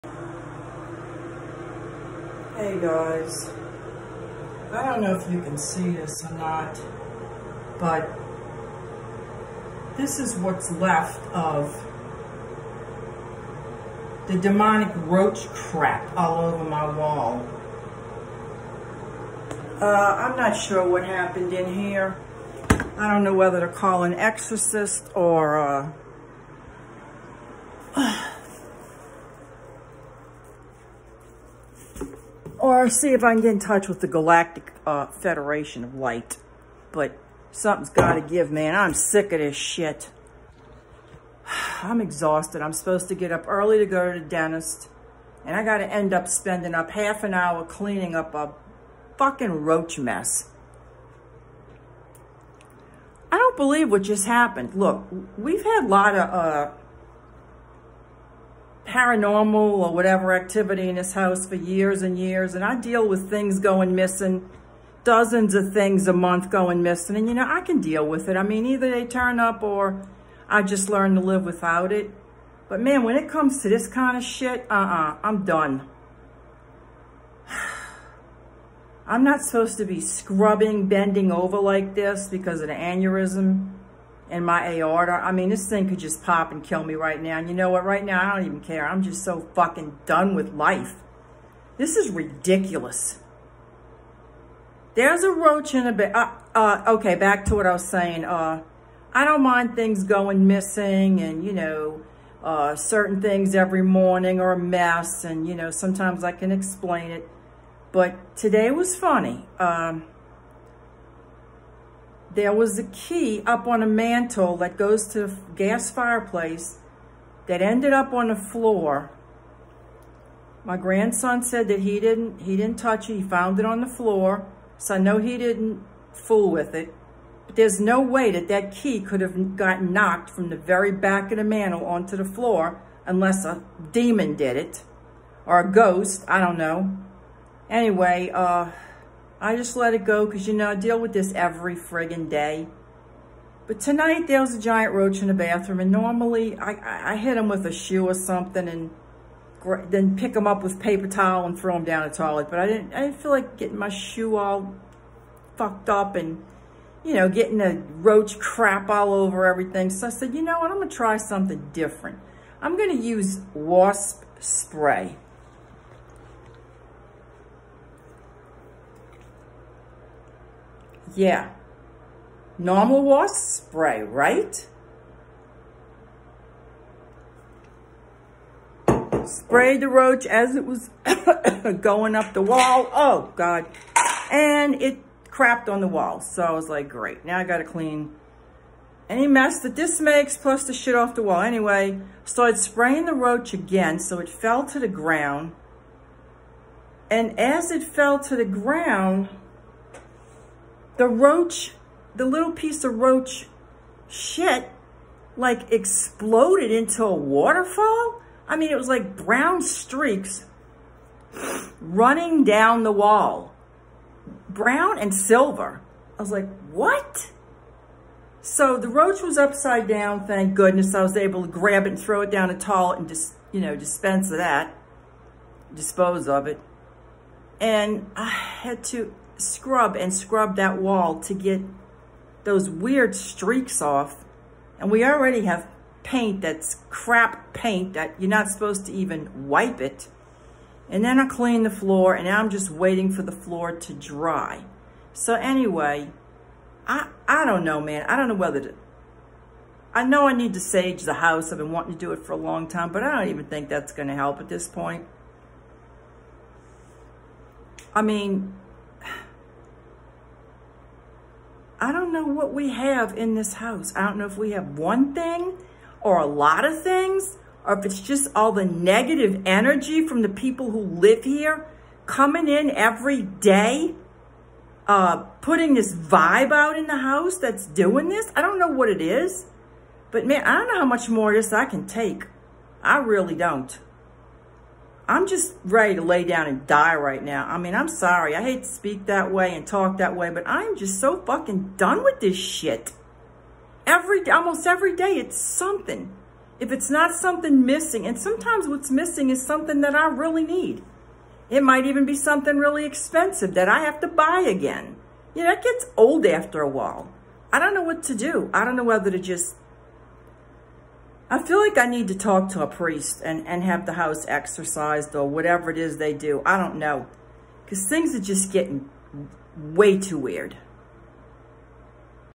Alien roach or demonic activity
Scary experience with a roach.